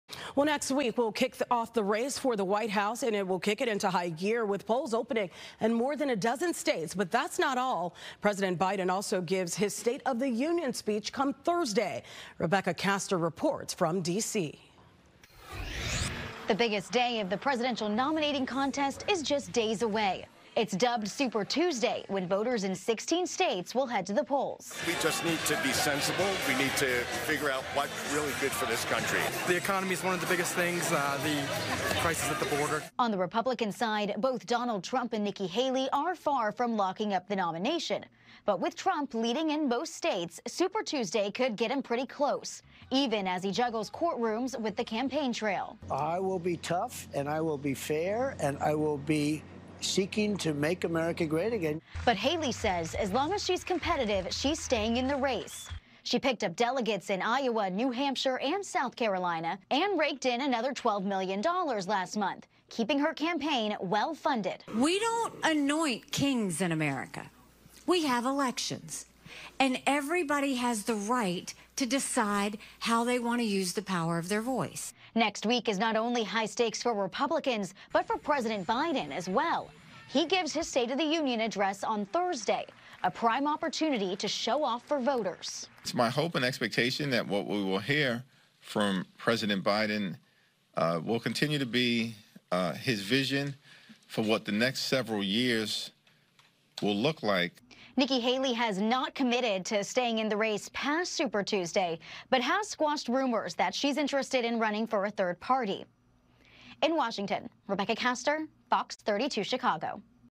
Super Tuesday just days away
Voters in 16 states will head to the polls next Tuesday.